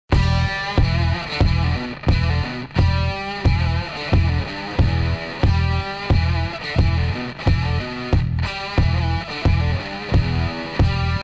Dog predicts results in CS:GO, Titan-Dignitas, 31-05-2014
Sorry than my dog bad predicted results in matches,
but Faceit is last chance :)

If my dog say wrong, I ends adds videos with Misia :(